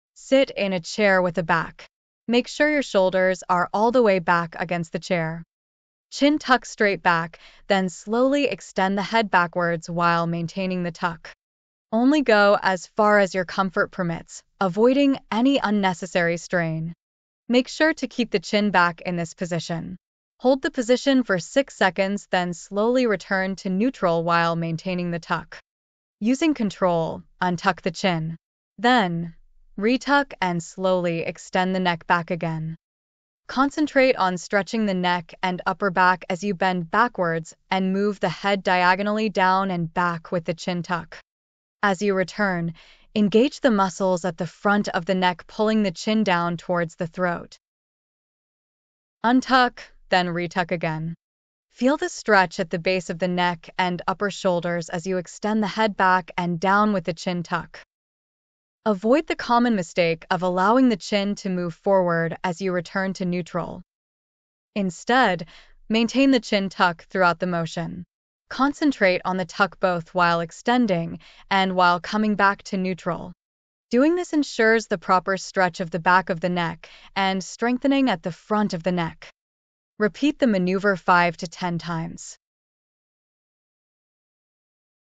Seated Neck Extension with Chintuck
Therapeutic exercise for neck pain extension plus chin tuck. Perform only under supervision of doctor or therapist.